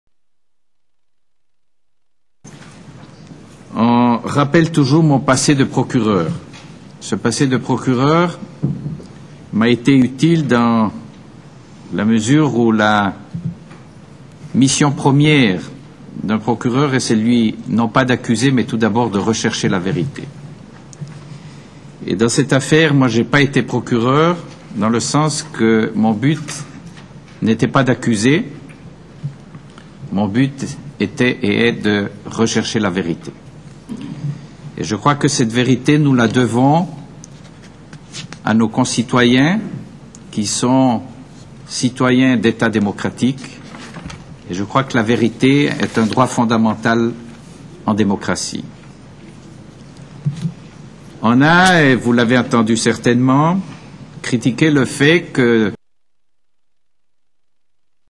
Le sénateur Dick Marty et sa défense du droit a la vérité
Monsieur Dick Marty:
En lisant les paroles de notre ancien Procureur Général de la Confédération, je me  demande et vous demande: si ces doivent être les principes appliqués aux criminels et personnes violentes, quels devraient être les principes appliqués a MA PERSONNE, un innocent?

«Un Etat de droit se doit de recourir à des moyens de droit pour lutter contre le terrorisme et la violence».
Vous trouverez ici, si vous no l'avez pas regardé encore, une section du discours de Monsieur Dick Marty, Ancien Procureur de la Confédération et Cantonal du Ticino, quand lui a parlé devant les journalistes pour commenter son investigation pour les vols illégaux de la CIA et les « renditions illégaux » dans le sol européen.
Toutefois, je vous encourage vivement a regarder le vidéo dans sa version complet et originale,